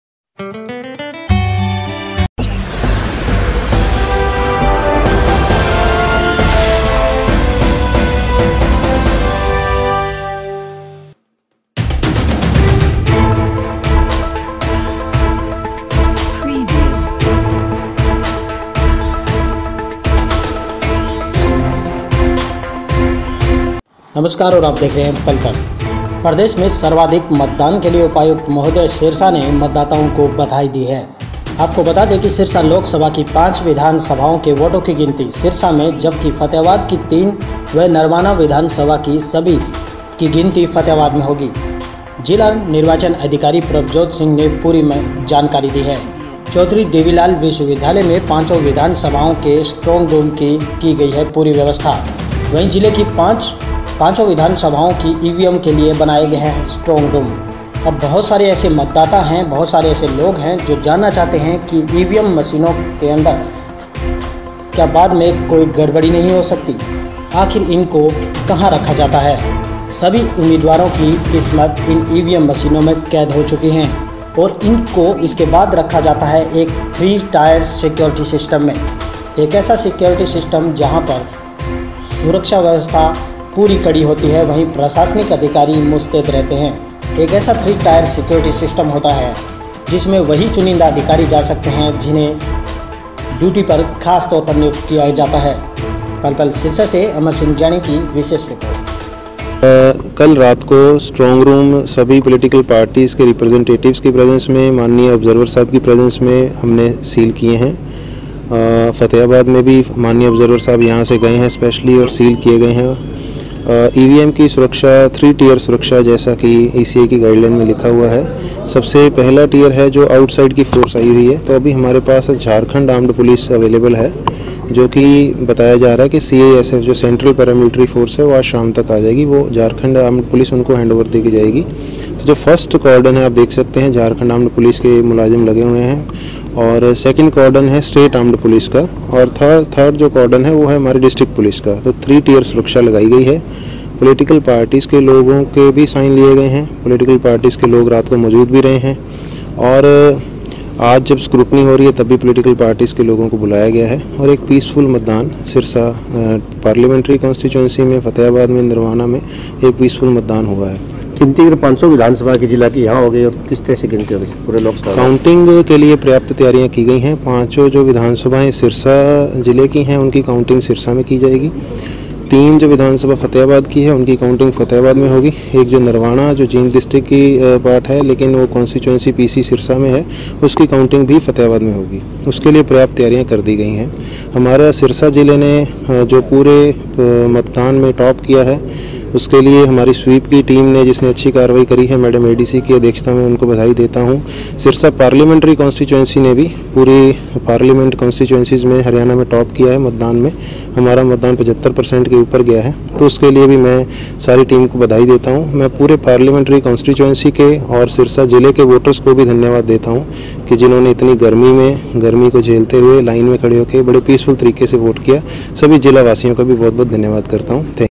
चुनावों# के बाद उम्मीदवारों की किस्मत का चिराग लिए कहाँ गई ''EVM'' मशीने
सिरसा -थ्री - टायर सिक्योरिटी सिस्टम में रखवाई गई ईवीएम।
- जिले की सभी पांचो विधान सभाओं की ईवीएम के लिए बनाए गए है स्ट्रांग रूम।
-चौधरी देवीलाल विश्विद्यालय में पांचो विधान सभाओं के स्ट्रांग रूम की की गई है व्यवस्था।
-जिला निर्वाचन अधिकारी प्रभजोत सिंह ने दी जानकारी।
-सिरसा लोक सभा की पांच विधान सभाओं के वोटों की गिनती सिरसा में जबकि फतेहाबाद की तीन व नरवाना विधान सभी की गिनती होगी फतेहाबाद में।
-प्रदेश में सर्वाधिक मतदान के लिए उपपयुक्त ने दी सिरसा के मतदाताओं को बधाई।